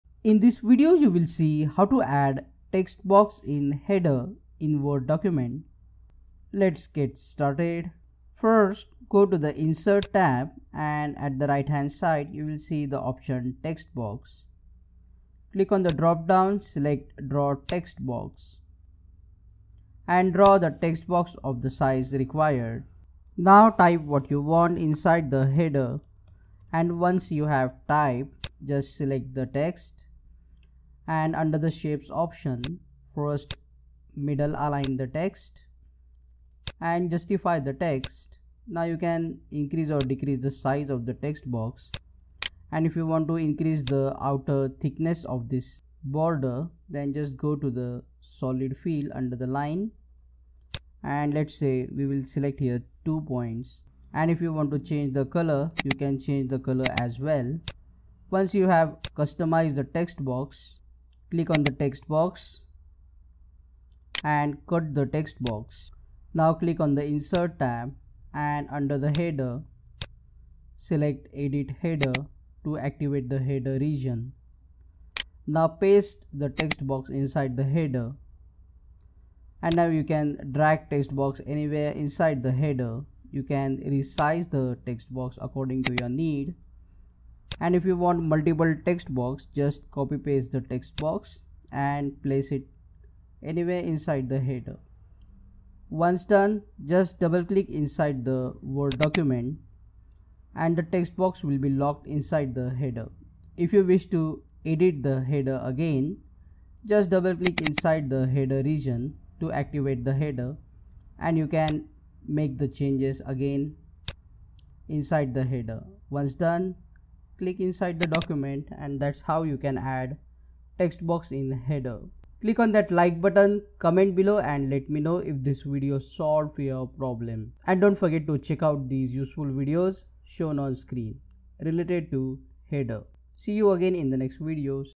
How To Add Text Box In Header In Word - [ SOLVED ]
Watch in this video How To Add Text Box In Header In Word document and move text box into header freely in Microsoft Word